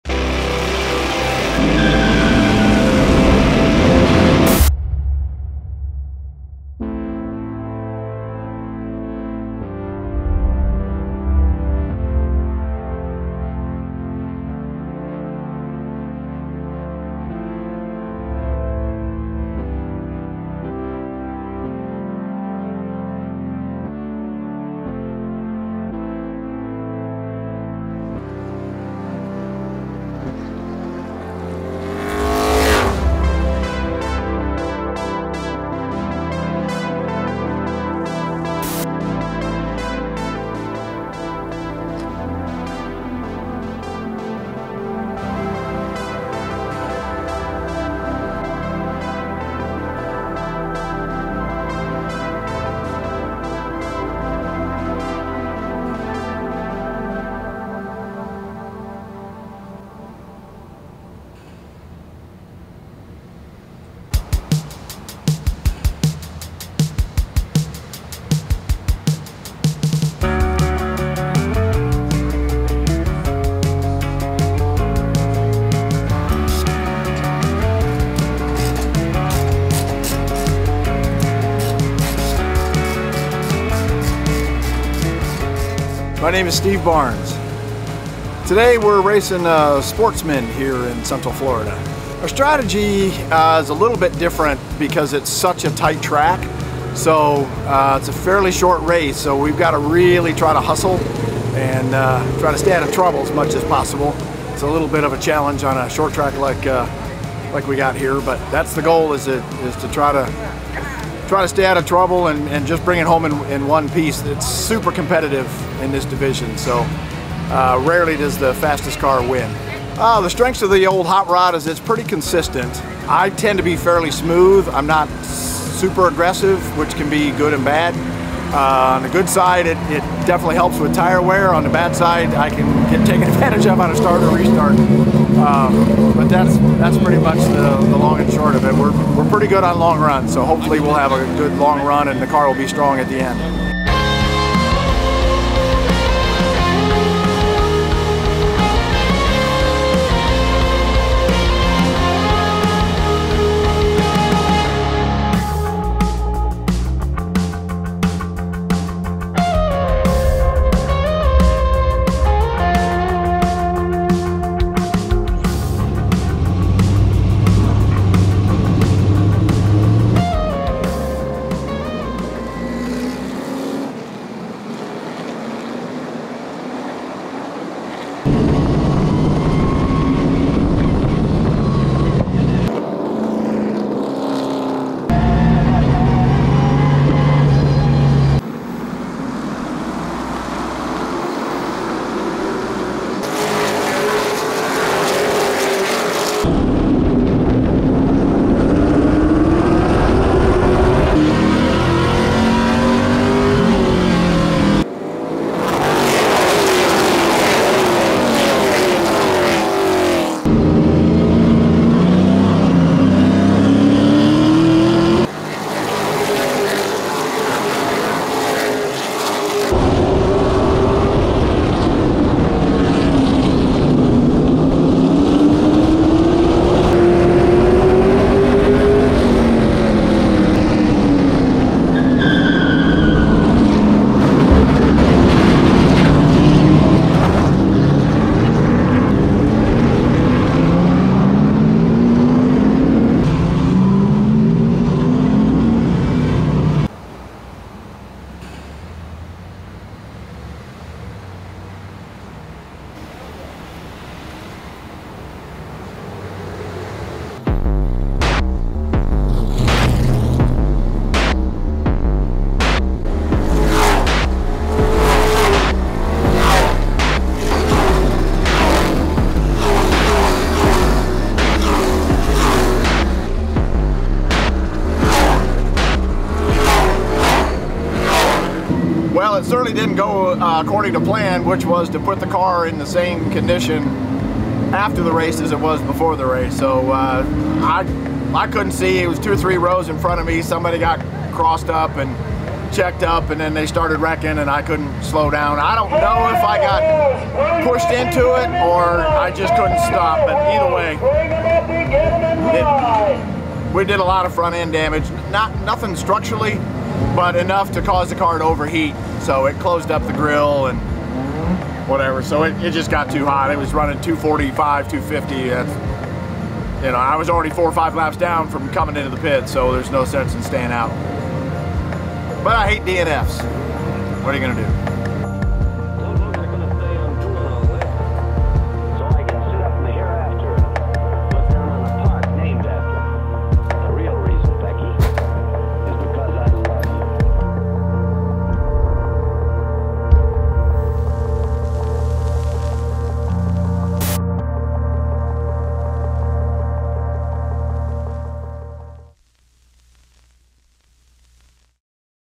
Sportsman | Circle Track Racing Mini-Documentary
Things don’t always go as planned, but a true sportsman keeps going.

“Sportsman” jumps into the exhilarating world of circle track racing. Steve Barnes is dealt a bad hand early in the race, but he is determined to finish it.

An Engle Digital Short
Director and Editor – Will Engle
Starring – Steve Barnes
Assistant Camera Operator – Jack Polly
Production Assistant – Hunter Osborne
Original Score – Luke Wilmoth

We are continuing to explore the mini-documentary format and are always looking for new stories to tell.

Shot using Sony A7IV and Sigma 24-70mm F2.8 Art lens.